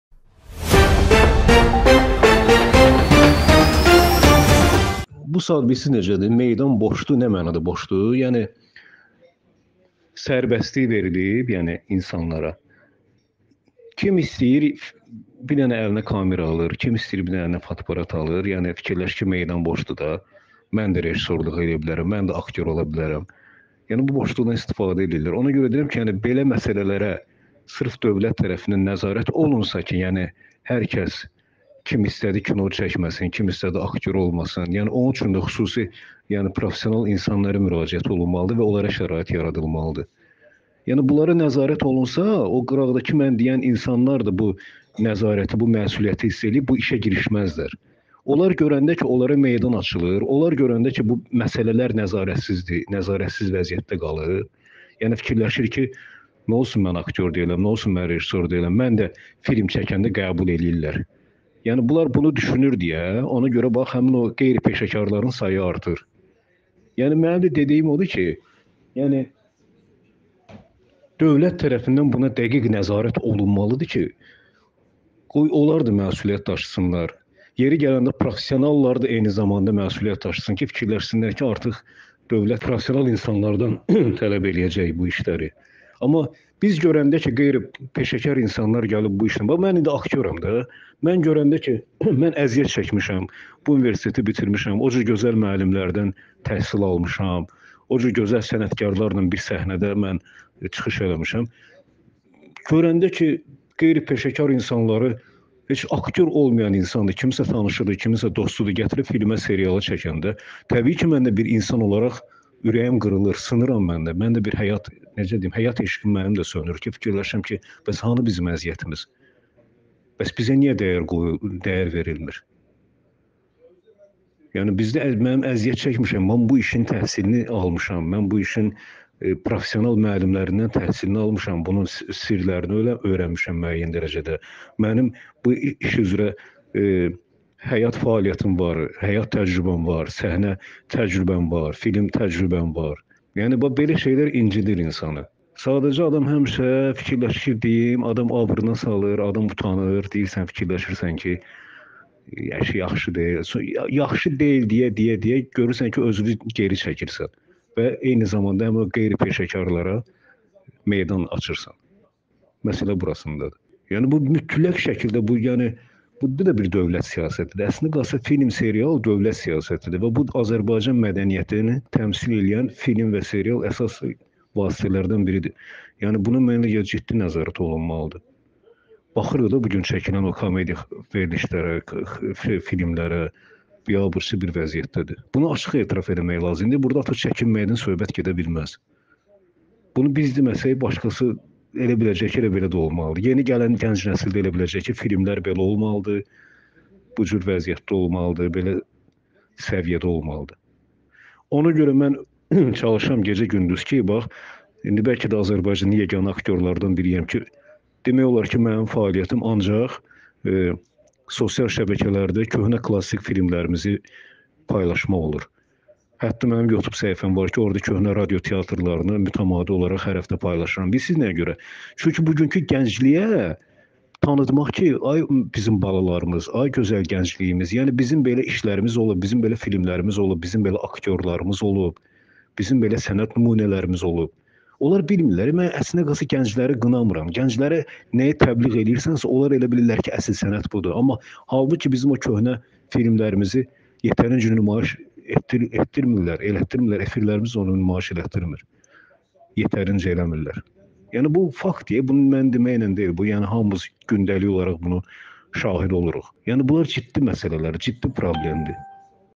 “Boş meydan”a axışan DİLETANTLAR: “Sənətdə peşəkarlara şərait yaradılmalı və...” - VİDEO
“Kim istəyir əlinə fotoaparat, kamera alır və fikirləşir ki, meydan boşdur, mən də rejissorluq edə, aktyor ola bilərəm”.

Bu sözləri “Yerli film və serial bazarının mövcud durumunu necə dəyərləndirirsiniz?” sualını cavablandıran Bələdiyyə Teatrının aktyoru, Əməkdar artist Elxan Rza Yenicag.az-a açıqlamasında deyib.

Aktyor qeyri-peşəkarların bu sahədən kənarlaşdırılması üçün dövlət tərəfindən nəzarətə ehtiyac olduğunu bildirib:

“İnsanlara sərbəstlik verilib, kim istəyir, əlinə fotoaparat, kamera alır və “onsuz da meydan boşdur, mən də rejissorluq edə, aktyor ola bilərəm”, – deyə düşünür. Yəni meydanın boş olmasından istifadə edirlər.

Belə məsələlərə dövlət tərəfindən nəzarət olmalıdır, peşəkar insanlara şərait yaradılmalıdır. Belə olsa, digər dediyim insanlar da bu işə girişməzlər”.